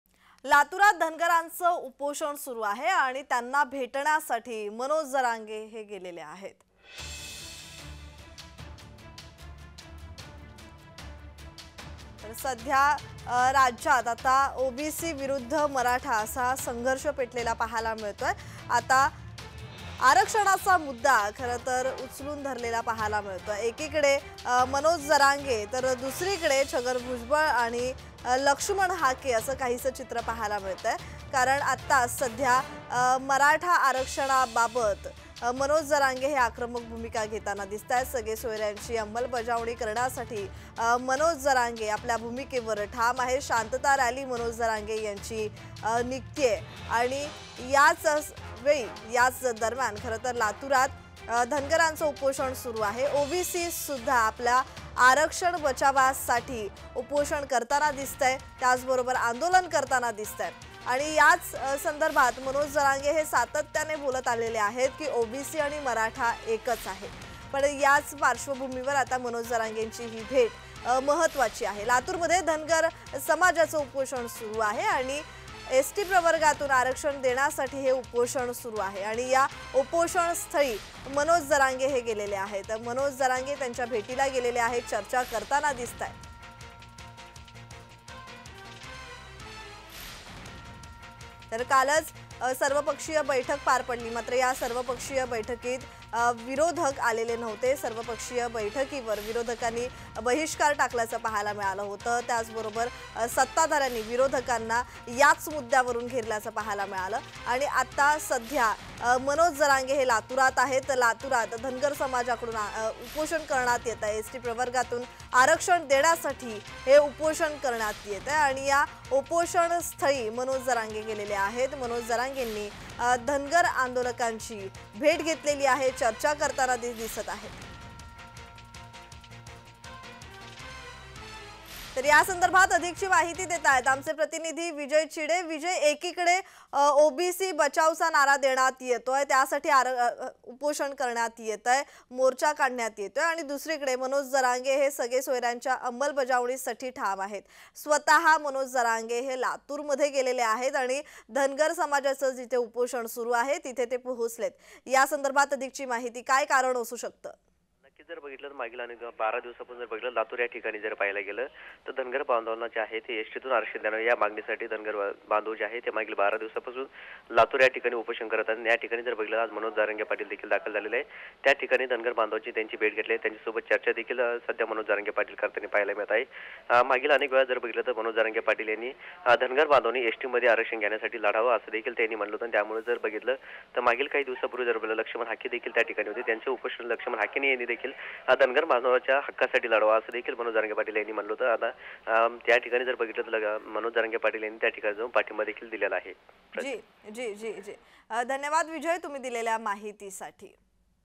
Manoj Jarange | जरांगेंनी घेतली धनगर उपोषणकर्त्यांची भेट | Marathi News
मनोज जरांगेंनी घेतली धनगर उपोषणकर्त्यांची भेट
लातूरमध्ये धनगर समाजाचं उपोषण
एसटी प्रवर्गातून आरक्षण देण्यासाठी उपोषण